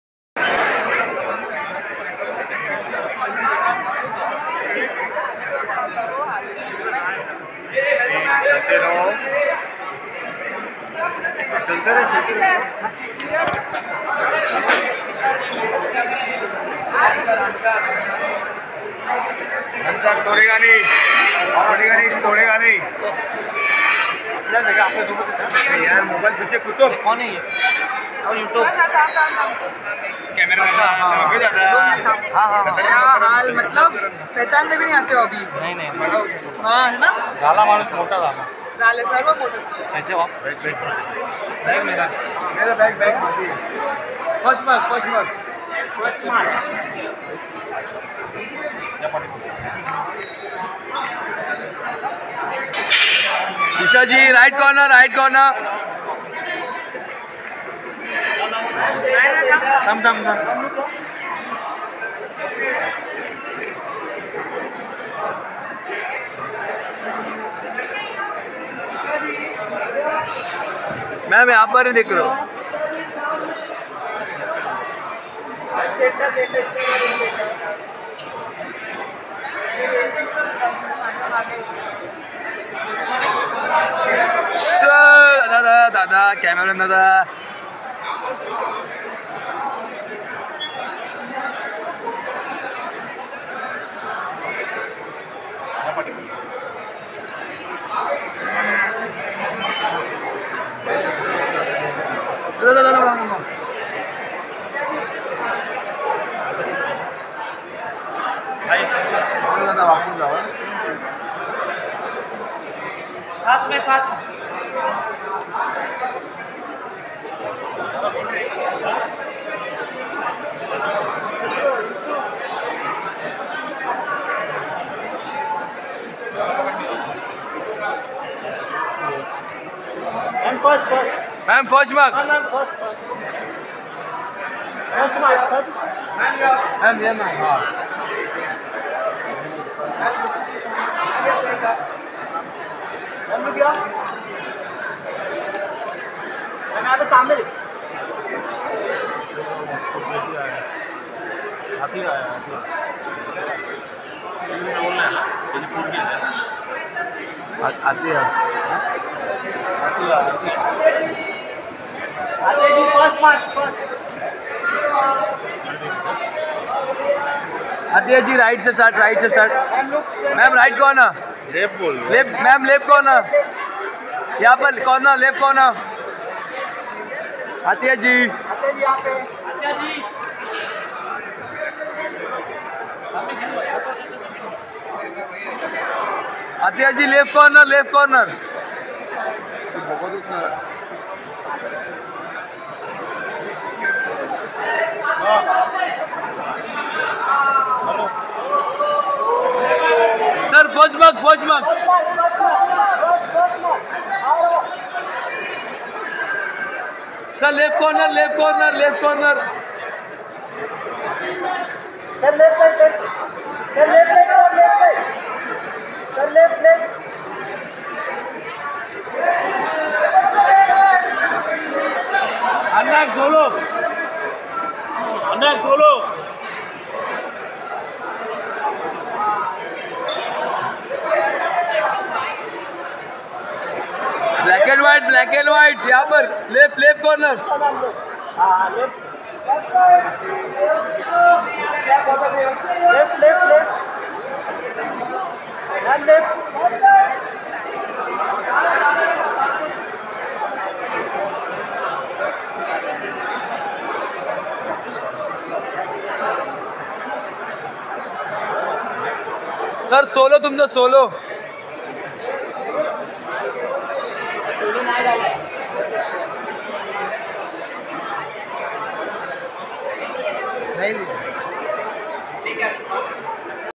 Esha Deol Stylish Entry| Sunil Shetty Athia Shetty Chahat at Hunter Screening
Esha Deol Stylish Entry| Sunil Shetty Athia Shetty Chahat at Hunter Screening viralzayn6 @viralzayn6